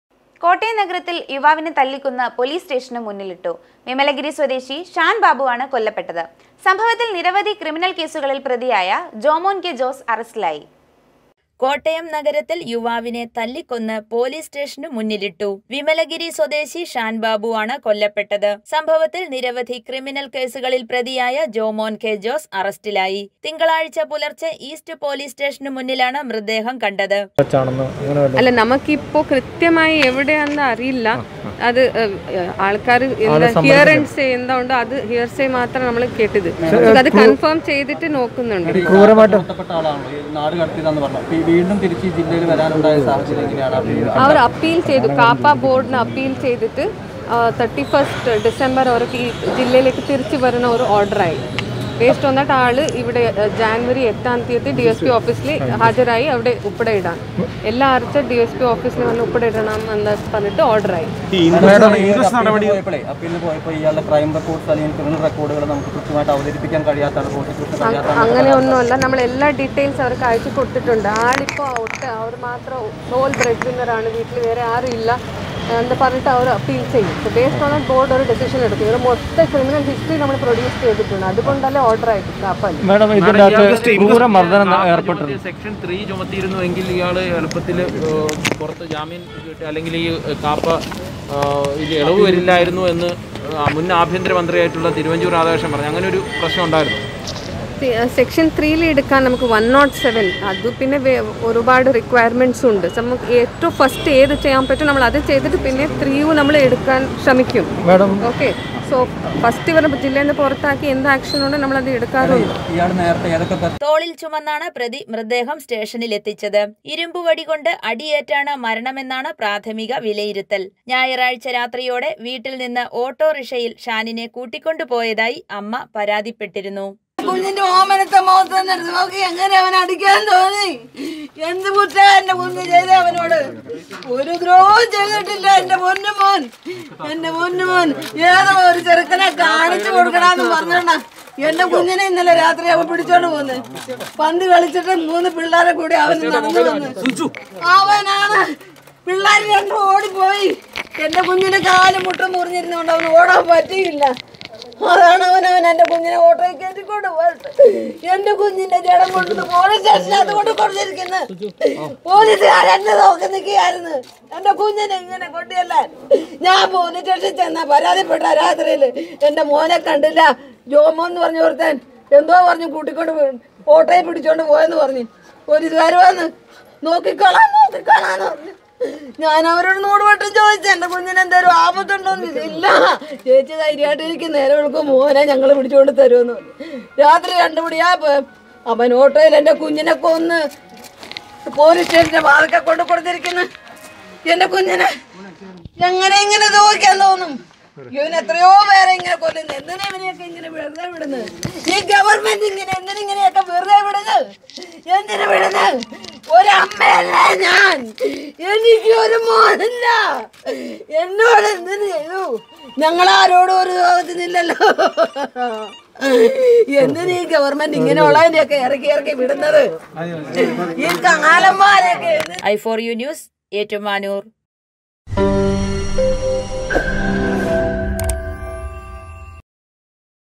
കോട്ടയം നഗരത്തില്‍ യുവാവിനെ തല്ലിക്കൊന്ന് പൊലീസ് സ്റ്റേഷന് മുന്നിലിട്ടു
കോട്ടയം നഗരത്തില്‍ യുവാവിനെ തല്ലിക്കൊന്ന് പൊലീസ് സ്റ്റേഷന് മുന്നിലിട്ടു. വിമലഗിരി സ്വദേശി ഷാന്‍ ബാബുവാണ് കൊല്ലപ്പെട്ടത്. സംഭവത്തില്‍ നിരവധി ക്രിമിനല്‍ കേസുകളില്‍ പ്രതിയായ ജോമോന്‍ കെ. ജോസ് അറസ്റ്റിലായി.